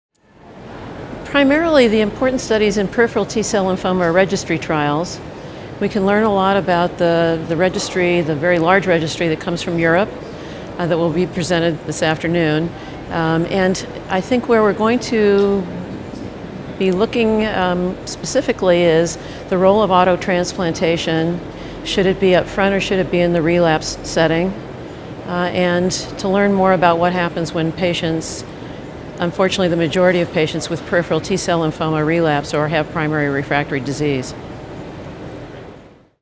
Lauren Pinter-Brown, MD, updates us on the important studies in T-cell lymphomas presented at ASH
Dr. Pinter-Brown, Professor of Medicine and Dermatology, UC Irvine, updates us on the important studies in T-cell lymphomas presented at ASH 2016